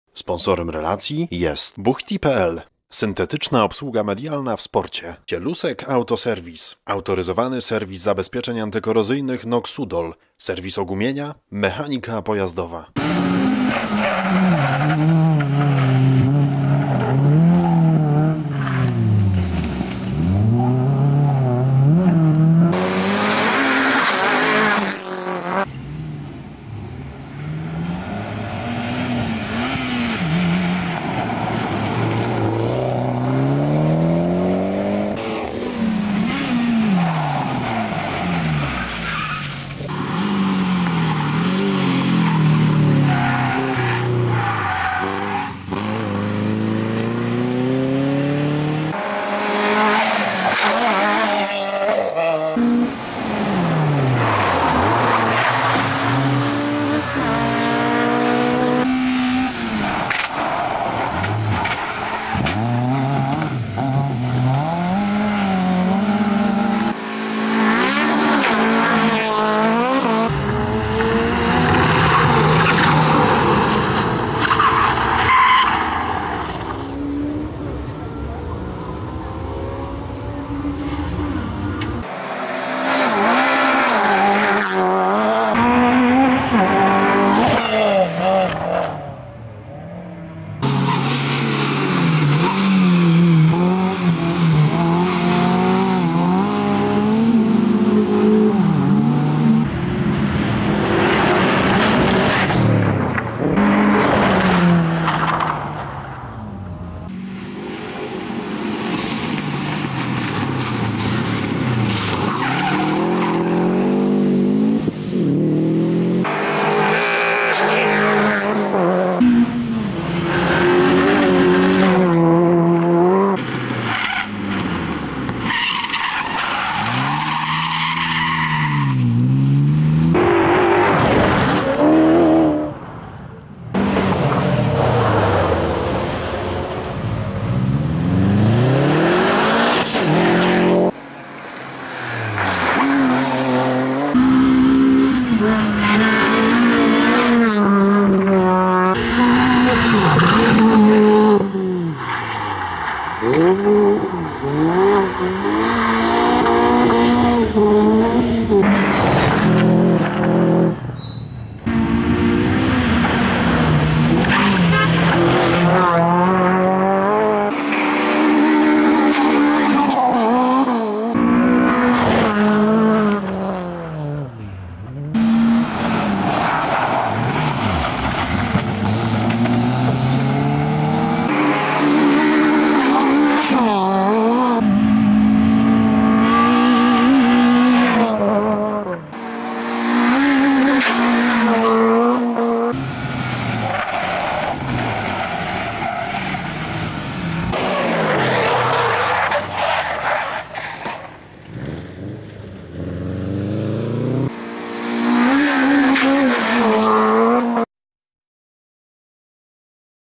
3 Runda SMT 2018 - Action&Crash by JVHD
3 Runda SMT 2018 - Action by JVHD
Samochodowe Mistrzostwa Tychów 2018 | AZT | Tor Fiata